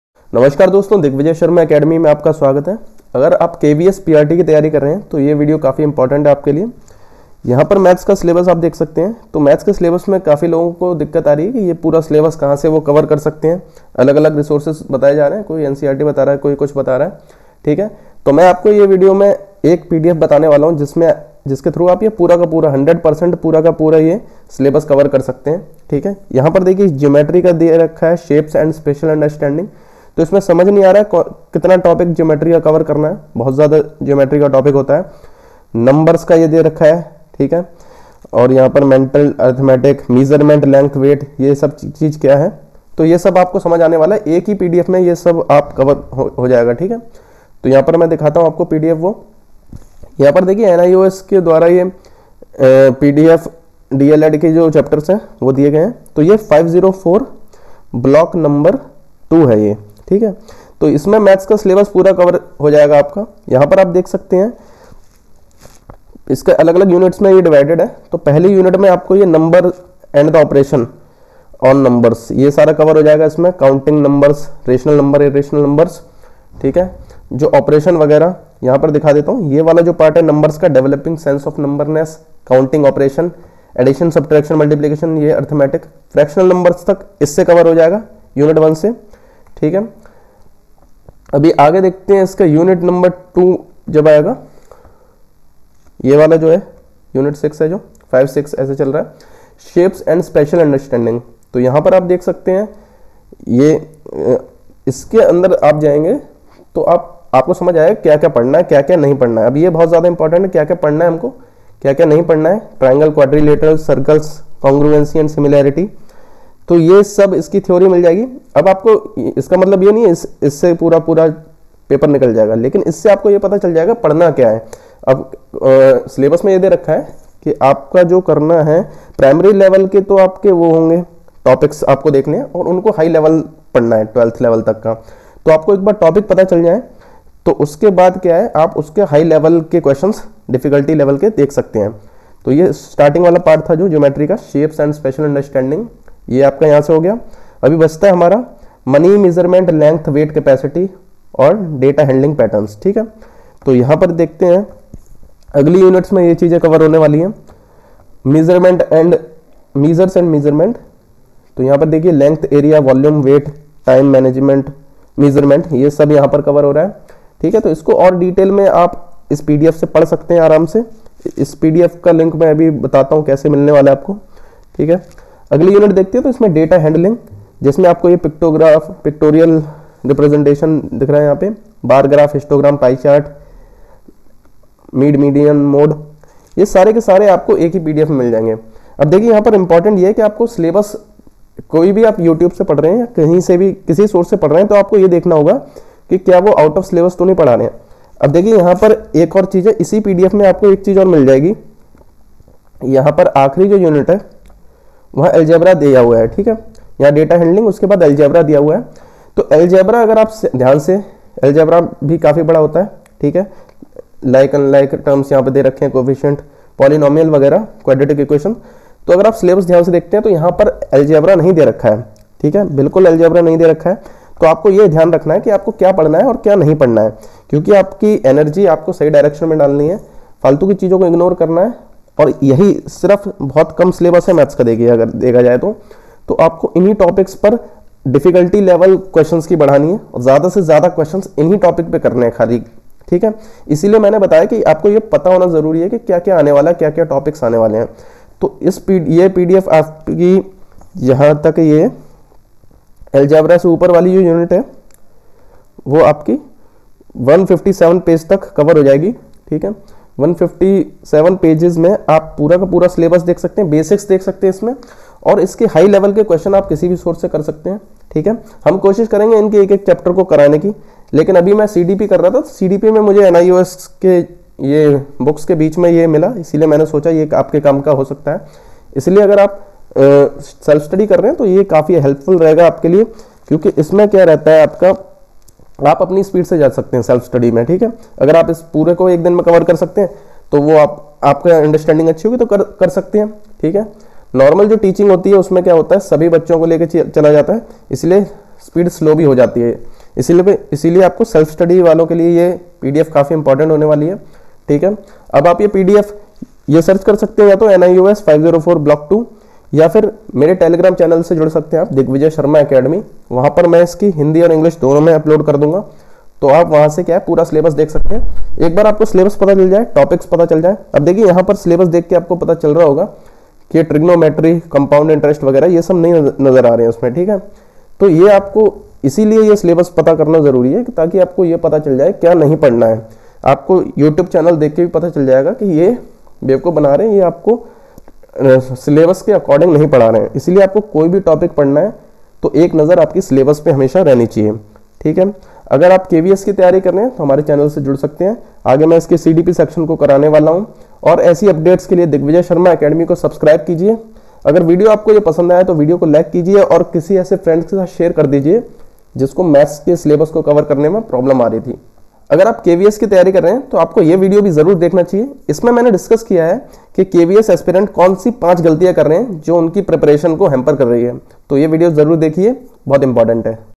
KVS PRT Complete Maths Section in 1 PDF
In this video we have discussed about strategy to complete entire maths section with one pdf. and also discussed about syllabus and strategy for each subject of kvs kvsvacancy2022 kvsvacancy kvsexam It's strategy and similarity with#aeesexam kvs stands for Kendriya vidyalaya Sangathan which is a very good organization for students aspiring to be in teaching field.